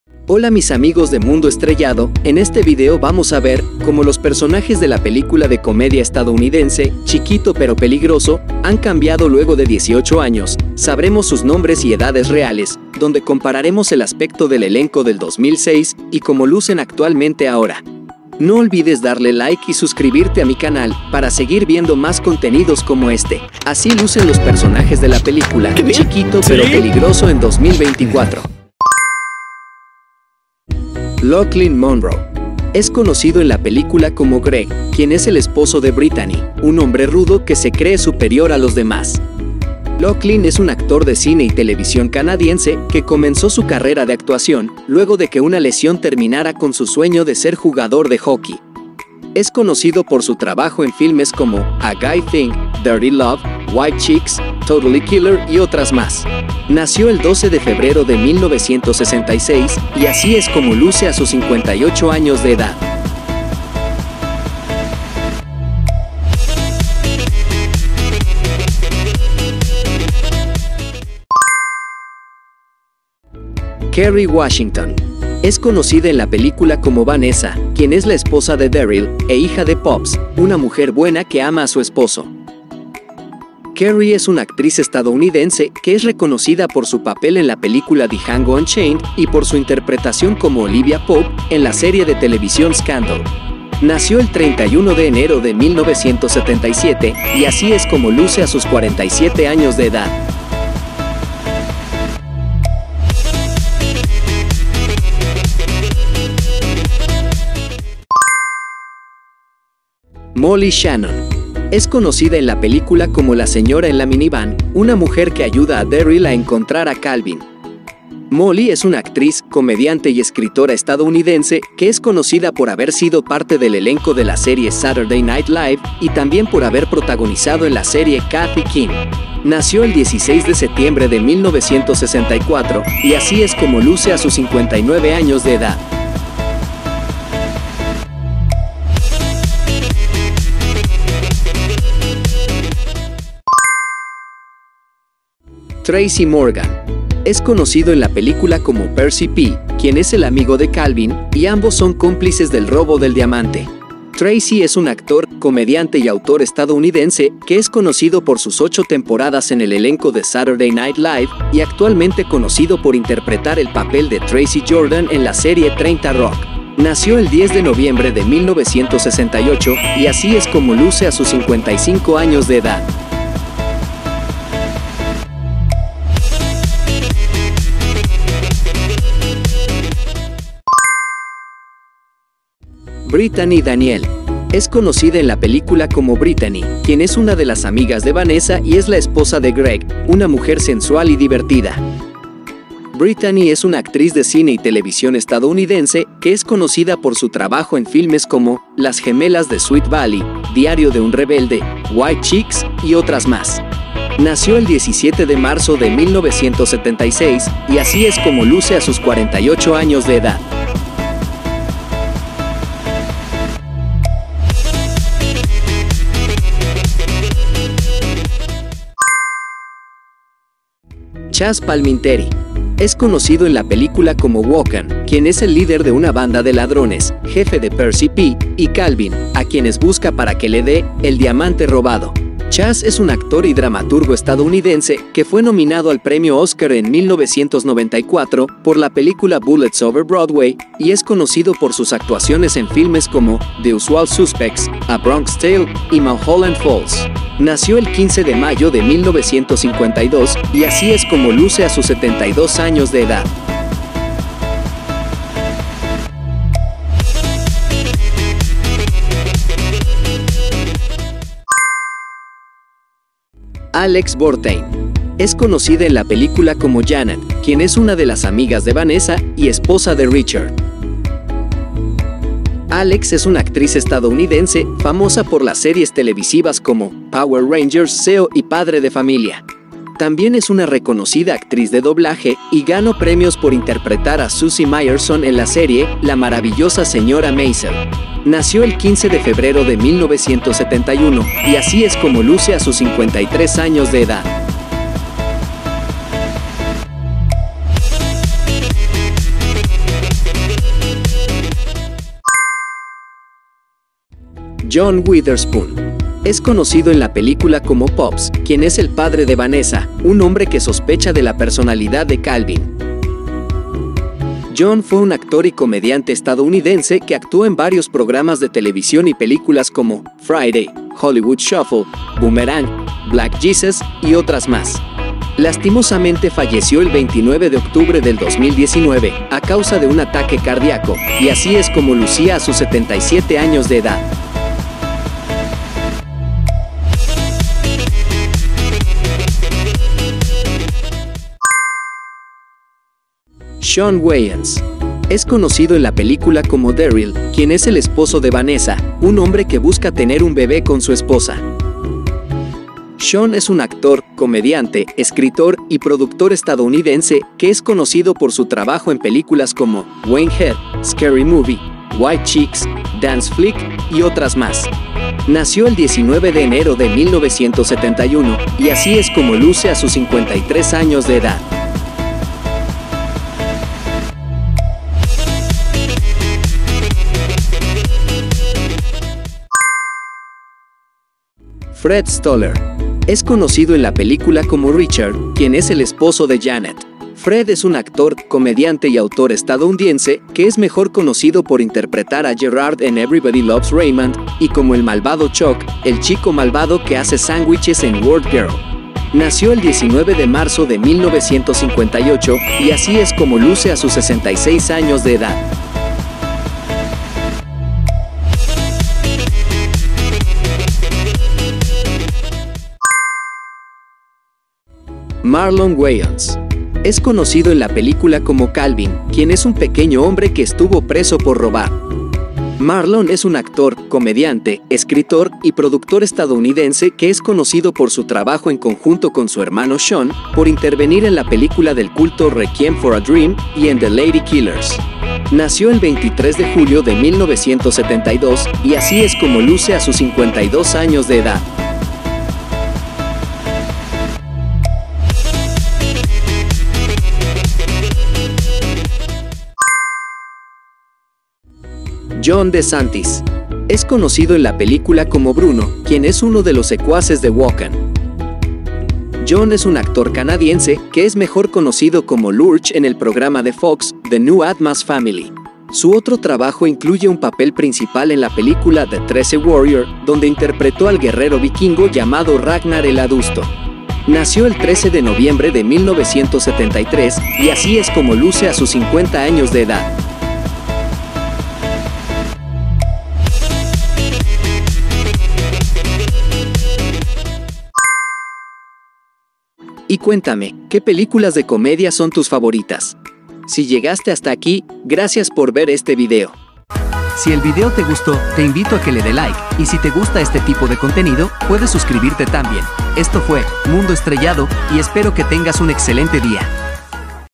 Así lucen los Actores de "Chiquito Pero Peligroso" en 2024 - Antes y Después
Chiquito Pero Peligroso (Little Man) ó también conocido como Pequeño pero matón, (2006) Elenco - Antes y después,  así lucen en la actualidad los actores y personajes de la película de comedia estadounidense, Chiquito Pero Peligroso (Little Man) ó también conocido como Pequeño pero matón, en 2024 ! Little Man Cast: Then and Now (2006 vs 2024).

Chiquito Pero Peligroso (Little Man) ó también conocido como Pequeño pero matón, es una película de comedia estadounidense dirigida por Keenen Ivory Wayans. Está protagonizada por Marlon Wayans, Shawn Wayans, Kerry Washington y Tracy Morgan. Se caracteriza por tener miembros del bastante largo, clan Wayans, mostrando a tres miembros del clan, cuyos dos son los protagonistas de la historia.

Reparto del Elenco de Chiquito pero peligroso:

Marlon Wayans como Calvin Sims.
Shawn Wayans como Darryl Edwards.
Kerry Washington como Vanessa Edwards.
John Witherspoon como Pops, el abuelo.
Brittany Daniel como Brittany.
Tracy Morgan como Percy.
Lochlyn Munro como Greg.
Chazz Palminteri como Walken.
Fred Stoller como Richard.
Alex Borstein como Janet.
Molly Shannon como Soccer Mom, y otras más.

¡GRACIAS!